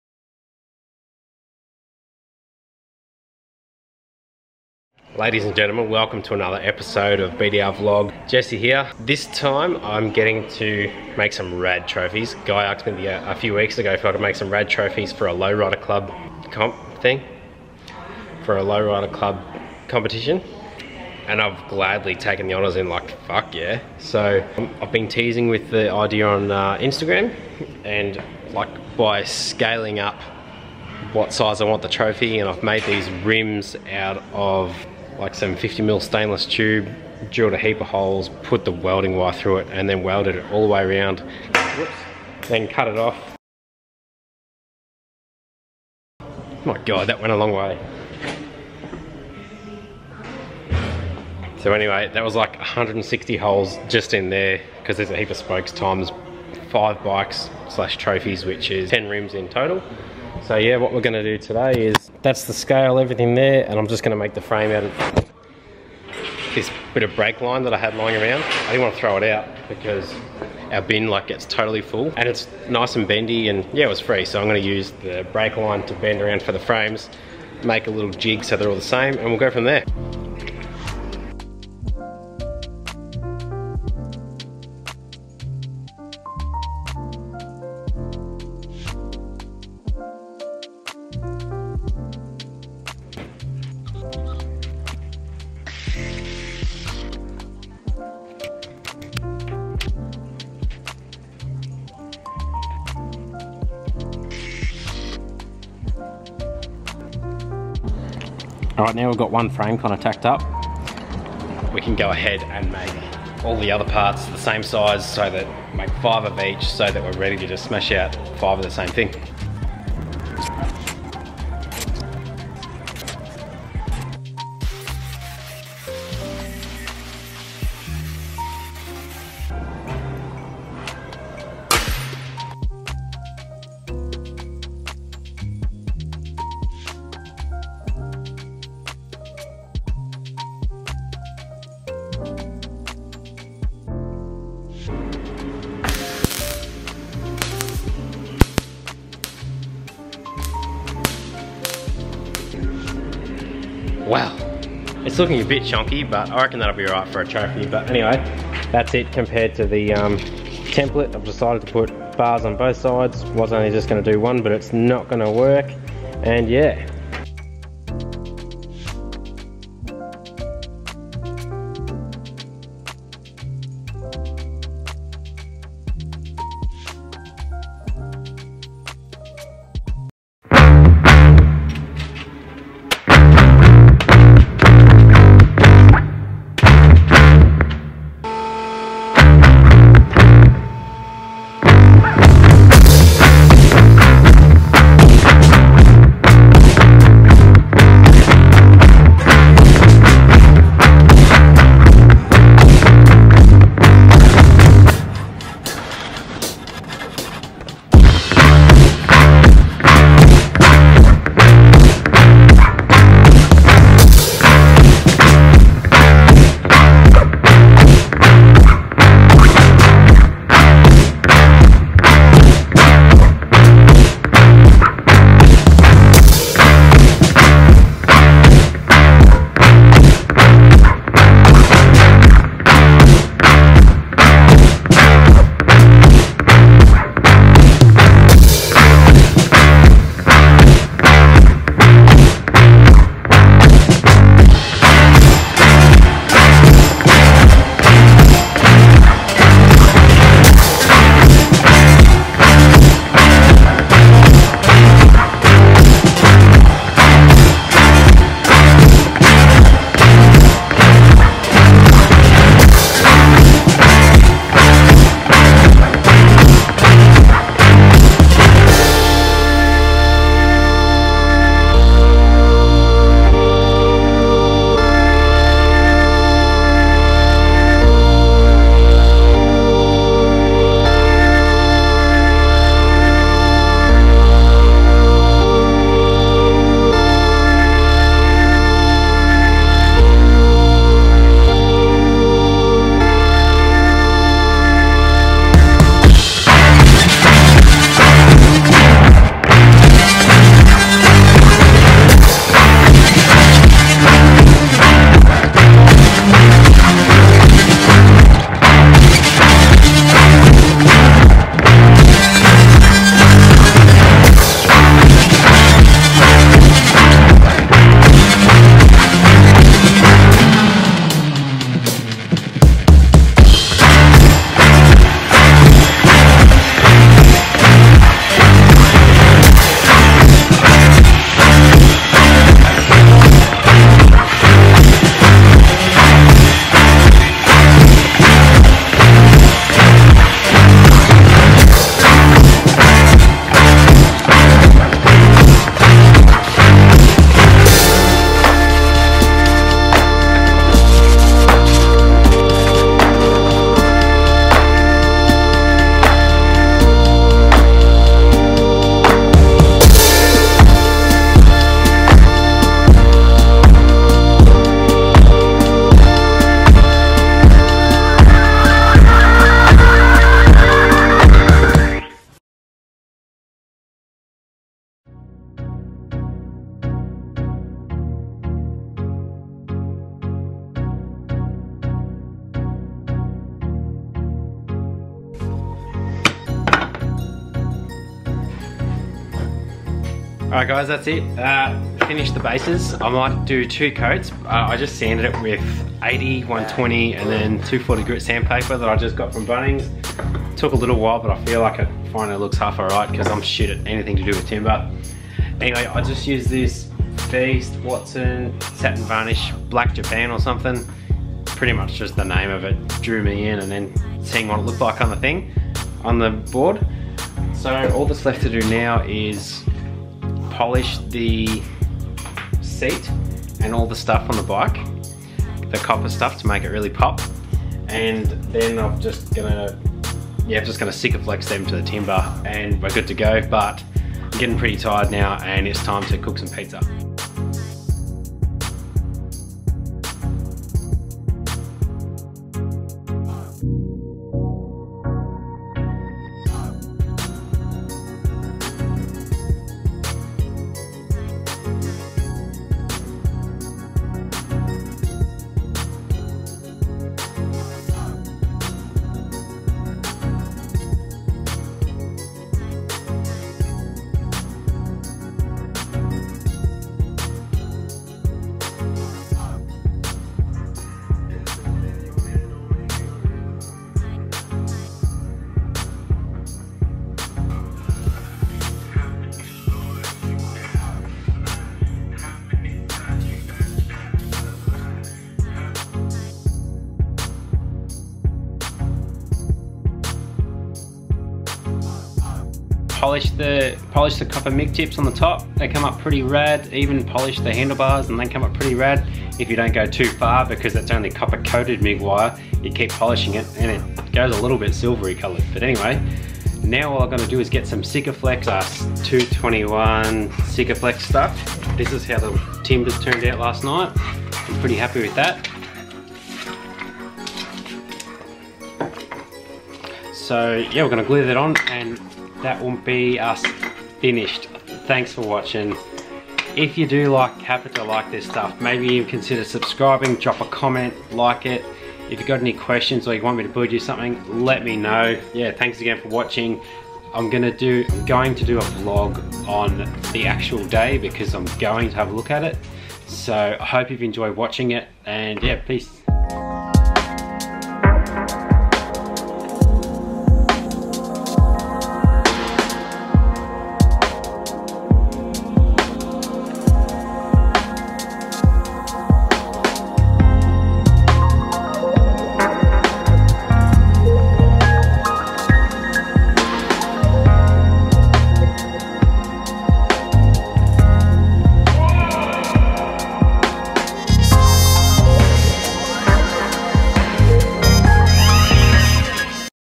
LOWRIDER TROPHY BUILD FROM SCRAP
This episode I make some lowrider trophies for the twisted customs lowrider club rat ran, which is held on the 6th April 2019.
I started with the wheels, which was stainless tube and mig wire.
Then the frames are bent up brake line from a ford escort.
the seats are crushed and polished mig tips.
handle bars filler wire.
bases are tasty oak from Bunnings
and the top tube is an old bandsaw blade.
The only thing I bought was the wood.